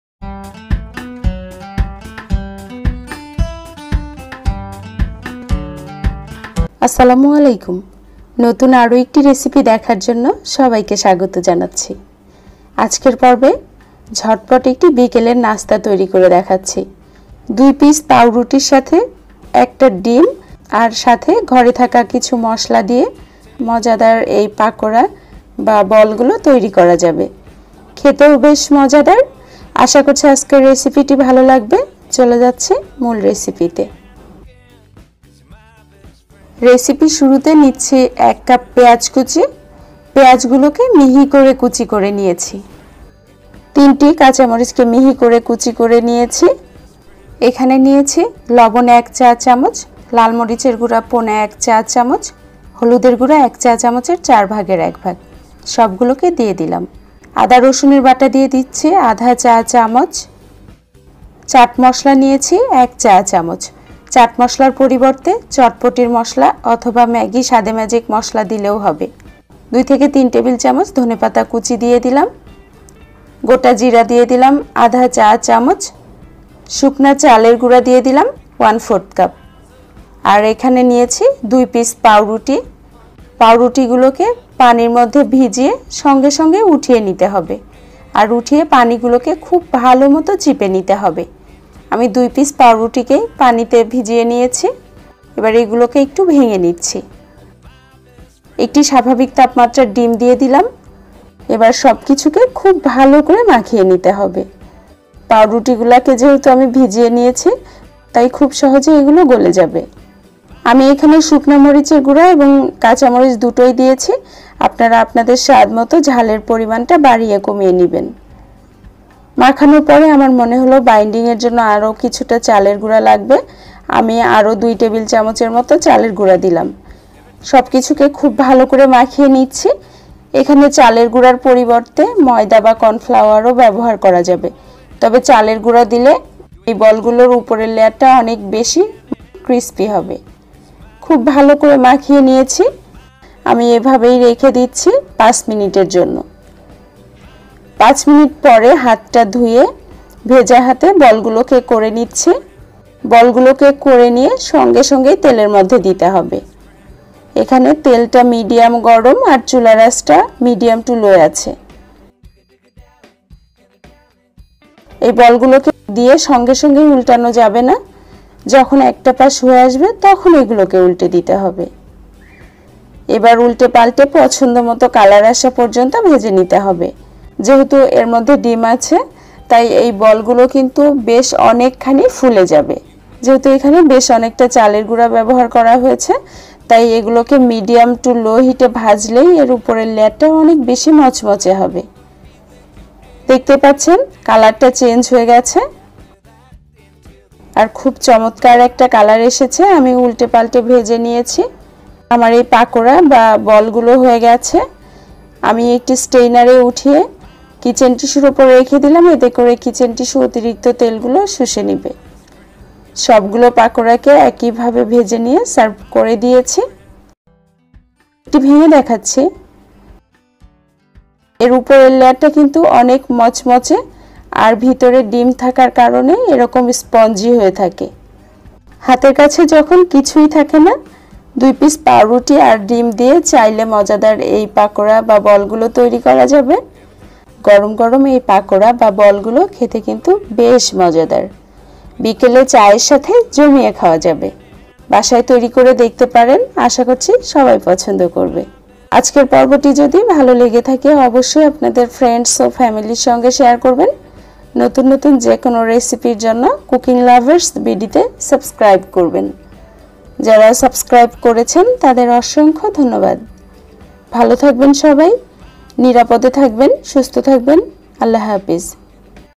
১টি ডিম ও পাউরুটির সাথে ঘরে থাকা অল্প কিছু উপকরণ দিয়ে মচমচে ও মজাদার বিকেলের নাস্তা।Bread pakora
১টি ডিম ও পাউরুটির সাথে ঘরে থাকা অল্প কিছু উপকরণ দিয়ে মচমচে ও মজাদার বিকেলের নাস্তা।Bread pakora